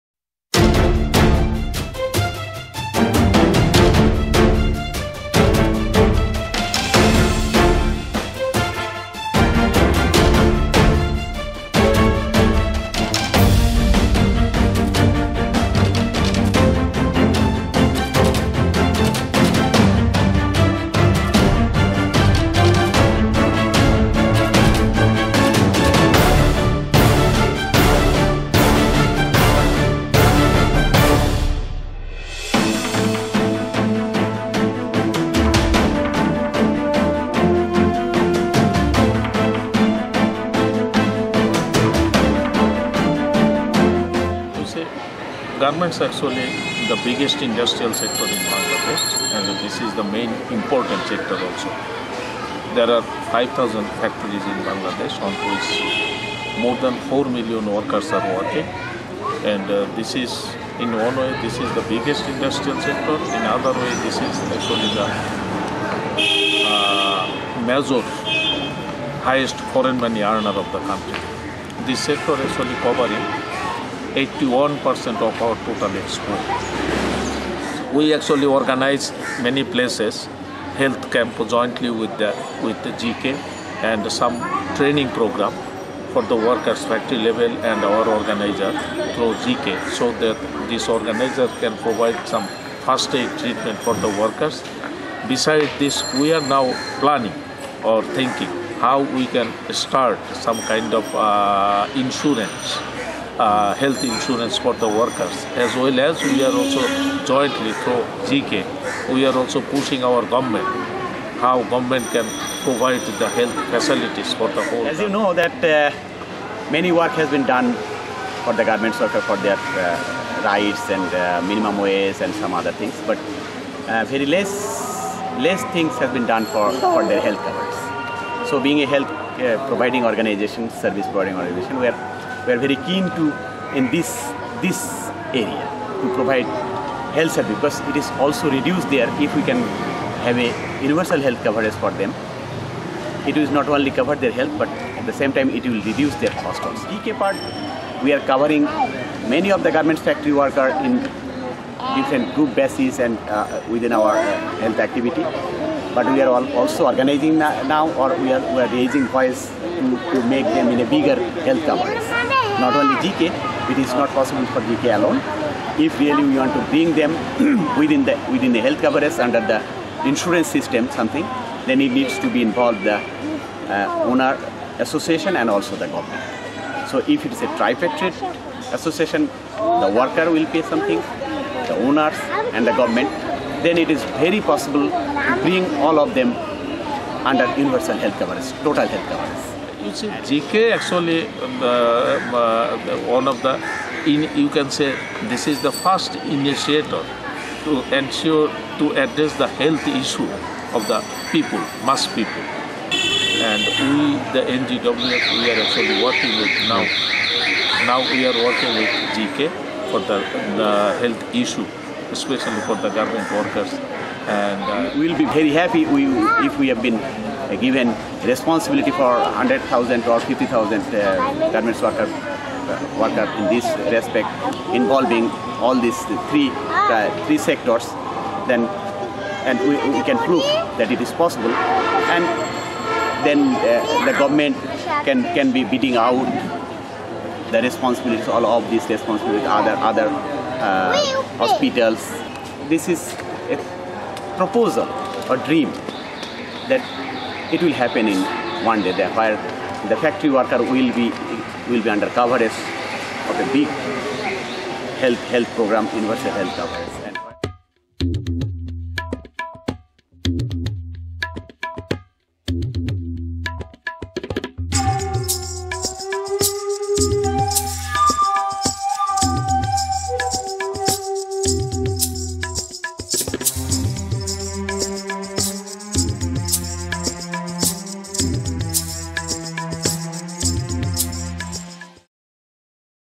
Universal Health coverage for garment workers - Synergy WSM Bangladesh
WSM's two Bangladesh partners, NGWF and GK are currently studying possibilities to set up a universal health coverage system for garment workers and how this could be achieved with involvement of state, employers and the trade unions.

However, it is very hard to bring the 46 different federations of the garment sector behind a common goal. Such a health coverage system would be complementary to the fight for an increase of the minimum wage, as garment workers would see their medical costs diminish through such coverage. In this video, Dr. Kadir from GK and Amin from NGWF speak about that possibility.
GK and NGWF are currently working together to offer health services at reduced costs to garment workers, medical aid to the victims of Rana Plaza and providing training to garment workers in Occupational Health and Safety and first aid.